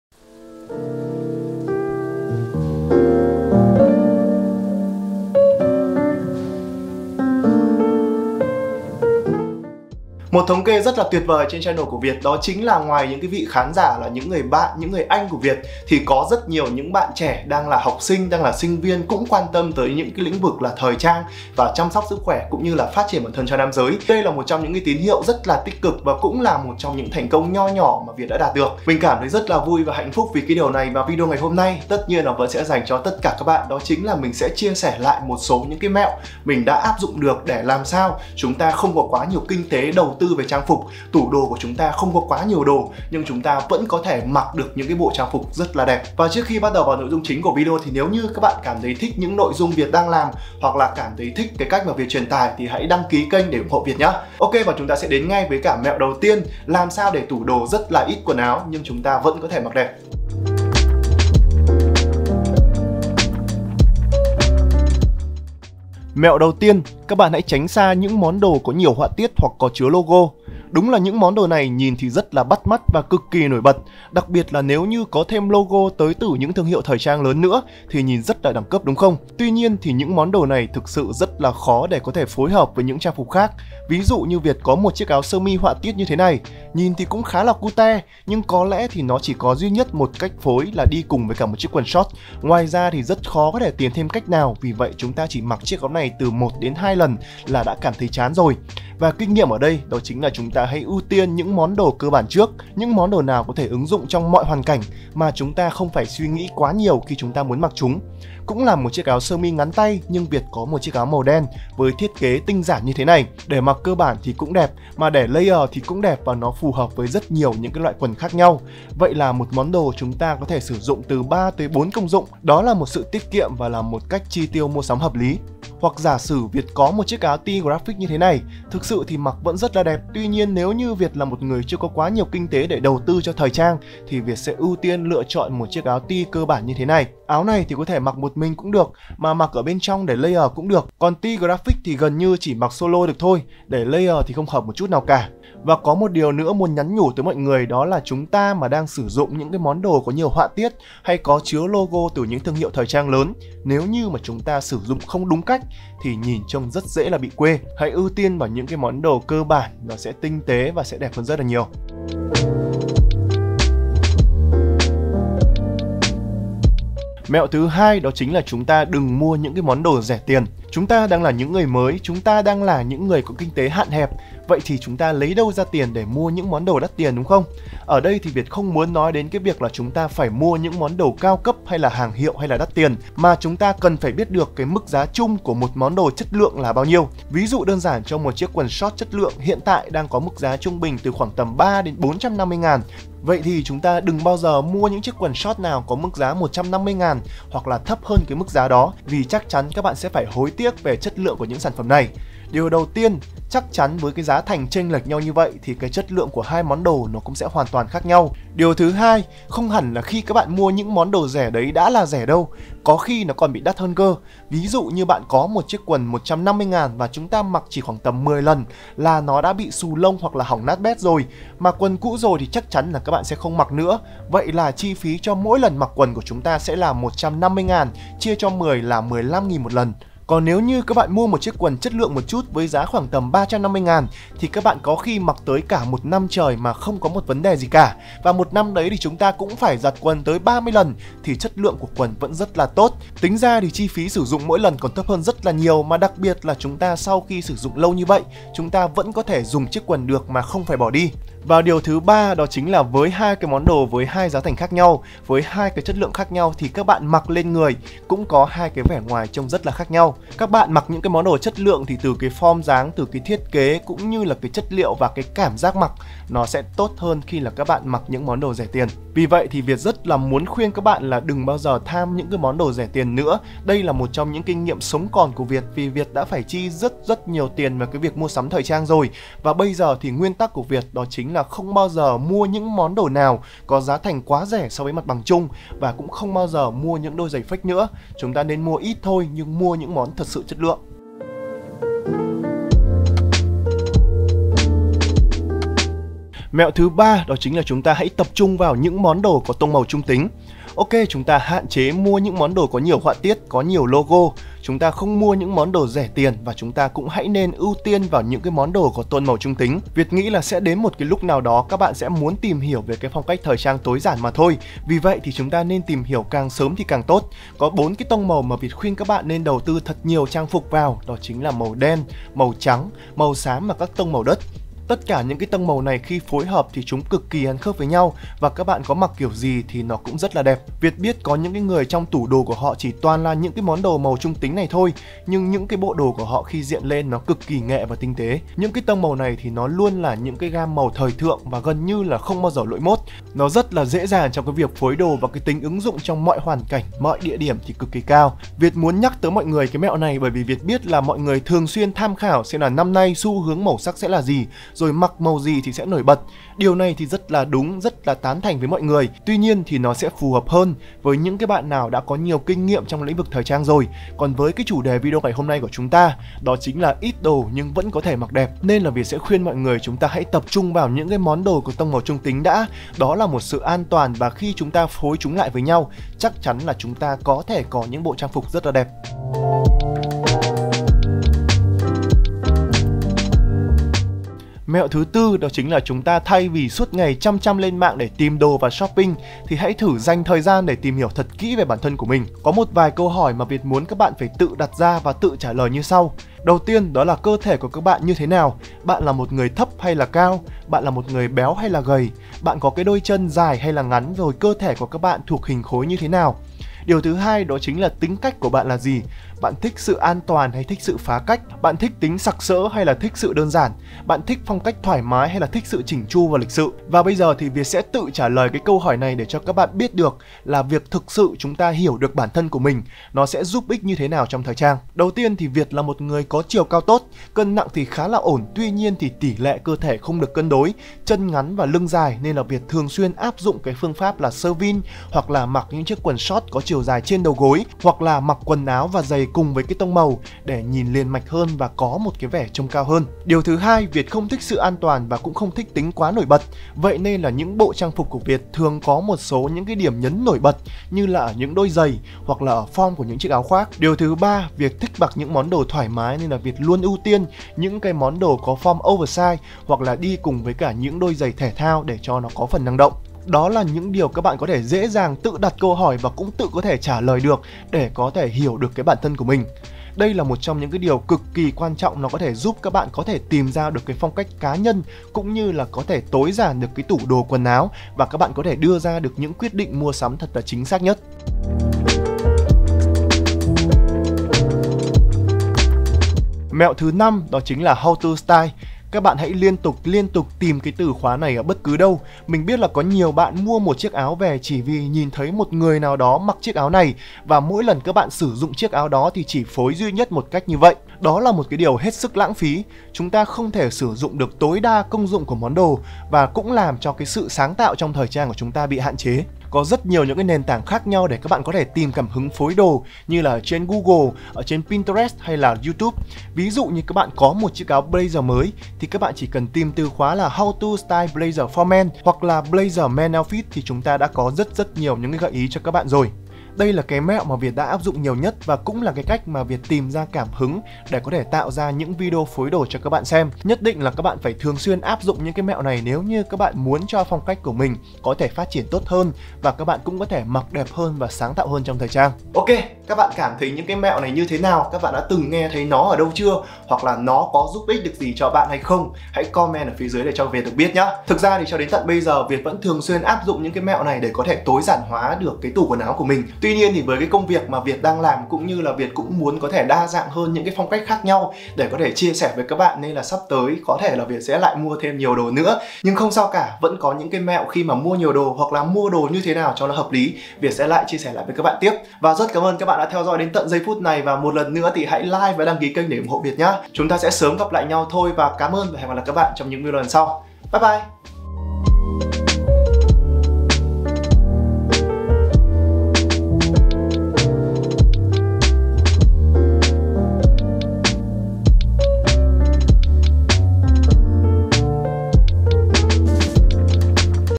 5 mẹo mặc đẹp mà không tốn kém, không cần nhiều đồ cho nam giới | Việt Nâu
5 mẹo mặc đẹp mà không tốn kém, không cần nhiều đồ cho nam giới | Việt Nâu